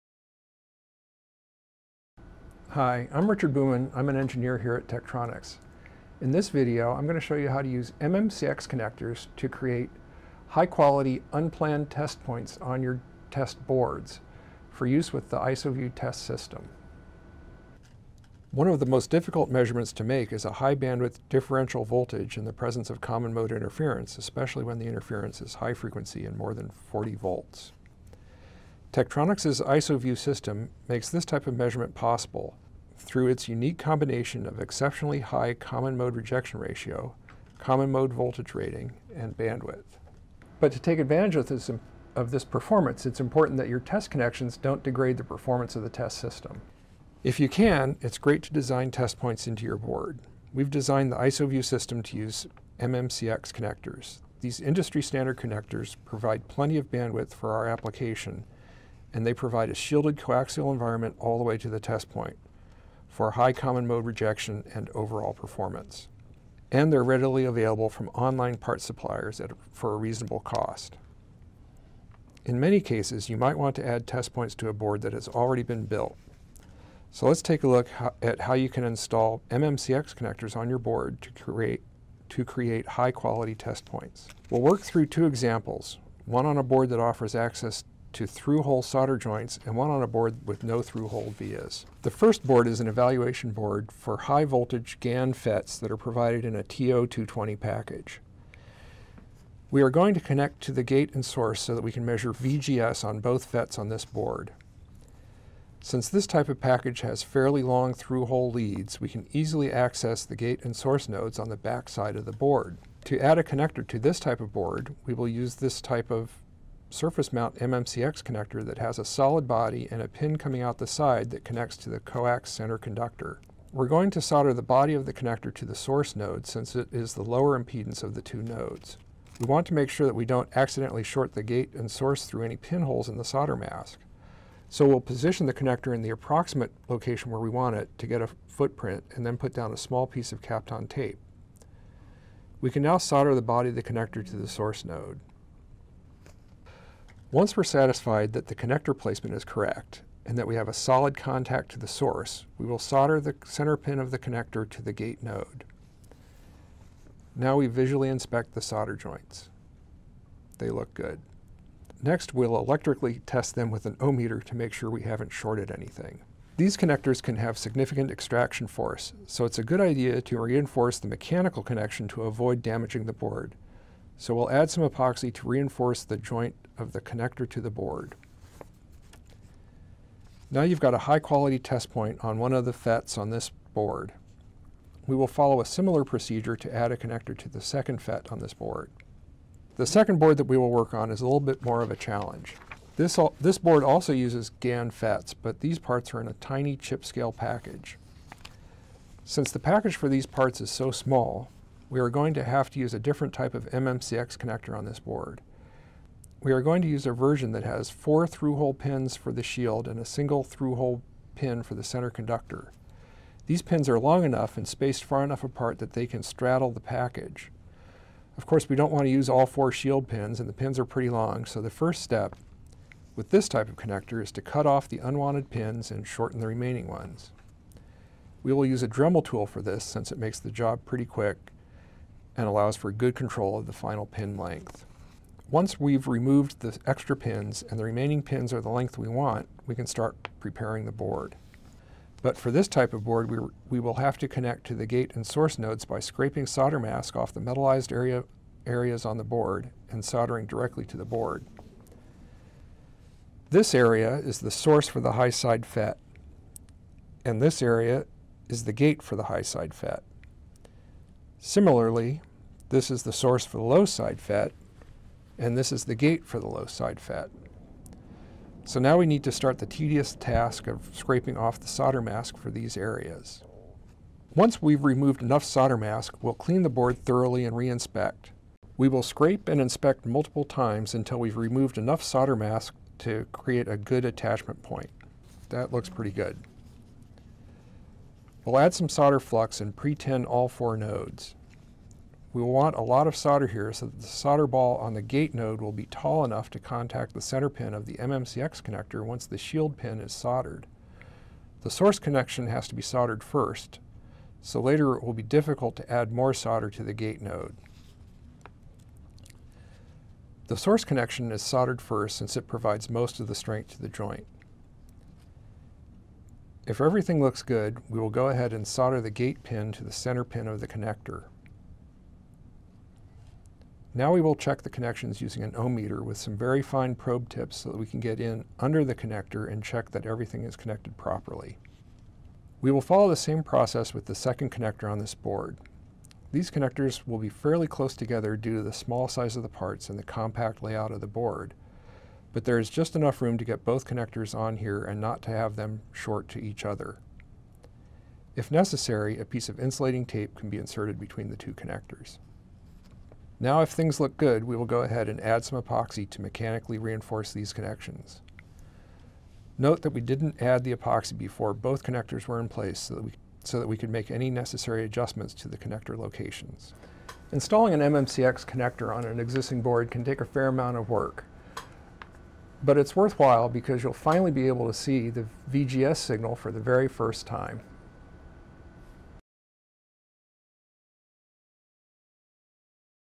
How to Use MMCX Connectors to Create Unplanned Test Points
In this video, we look at a measurement that hasn't been possible to make until now: High-side and low-side VGS measurements on a half bridge switching circuit, which uses two eGaN FETs as the switches.